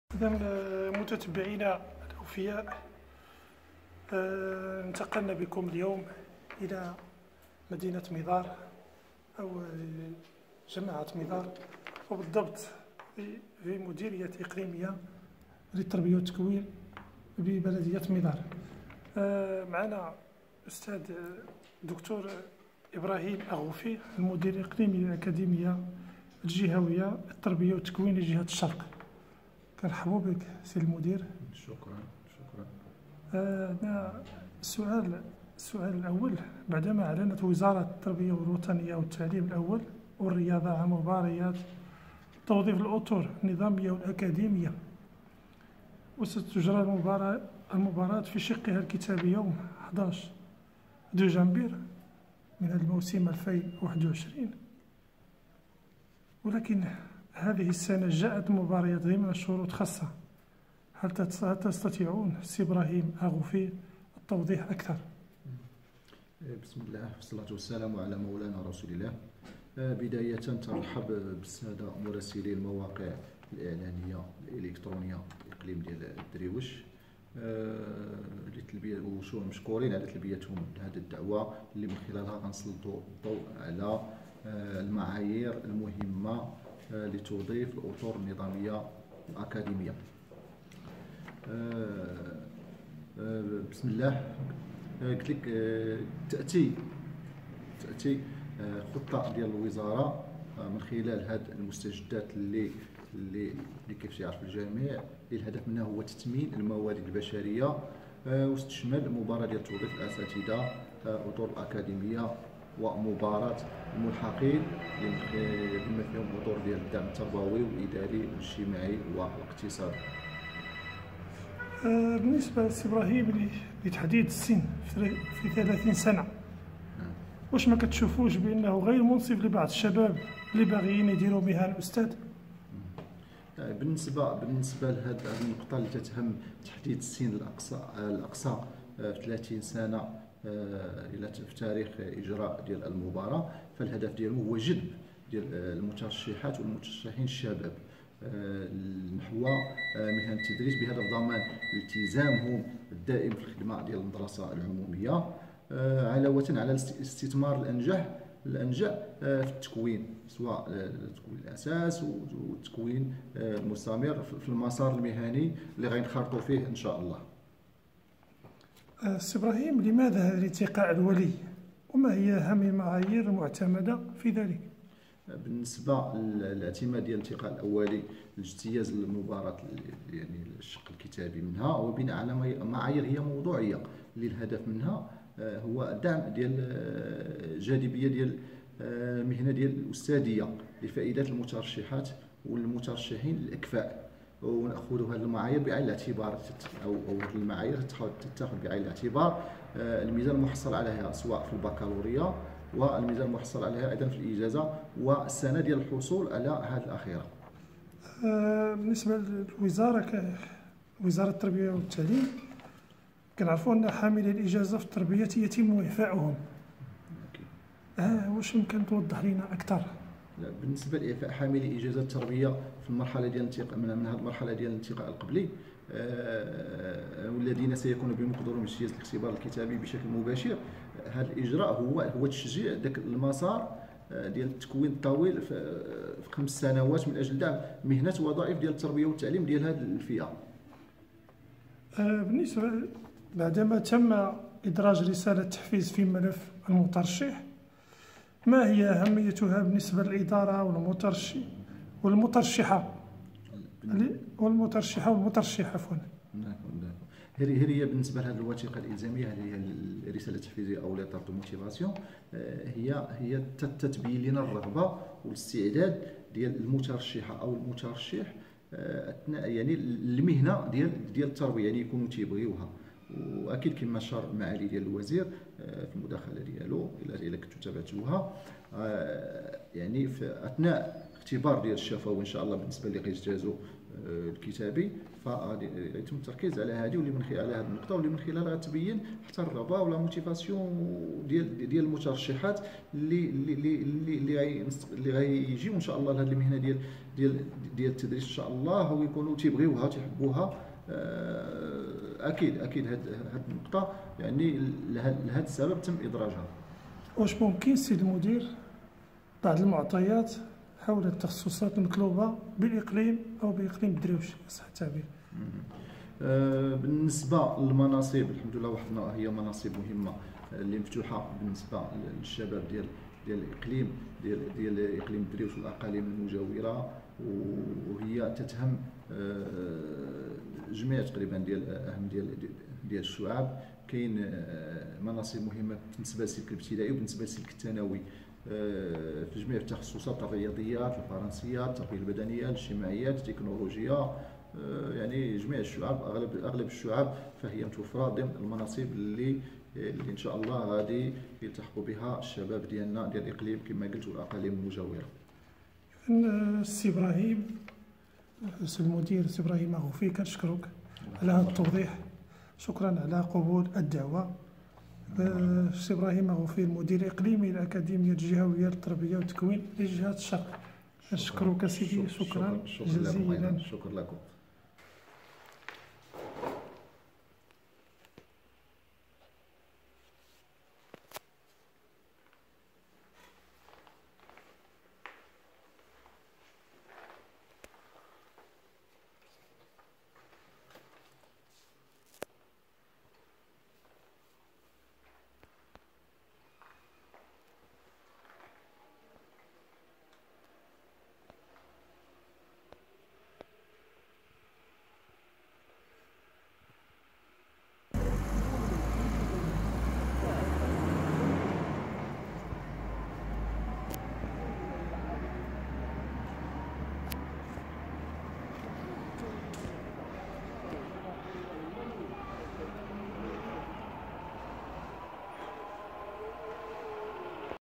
مدير مديرية اقليم الدريوش يوضح الاجرئات وشروط مبارة التعليم
حقوق الانسان بالريف